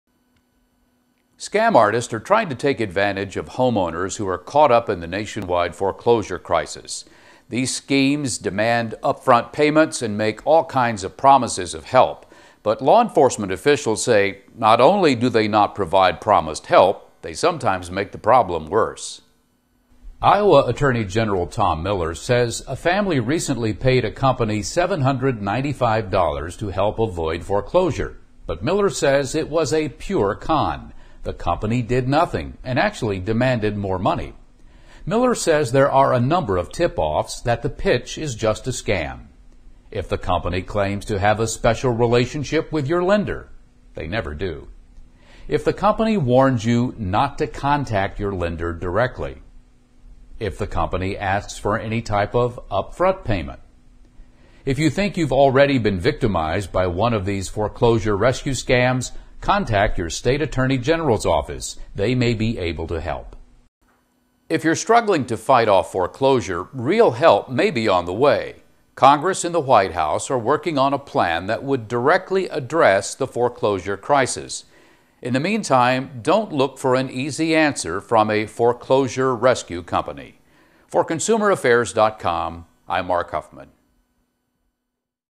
Avoid foreclosure rescue scams
ConsumerAffairs.com's Mark Huffman offers tips for identifying the foreclosure rescue scam.